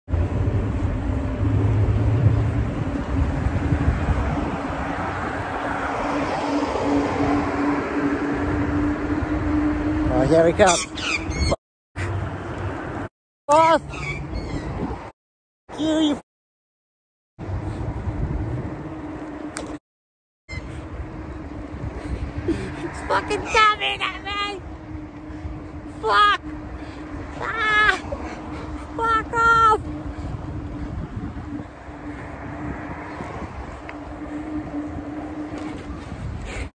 Attacked by magpie on my Torque
Just minding my own business . Then this bugger attacks me . He got me on the way there . So knew he would on the way back 😂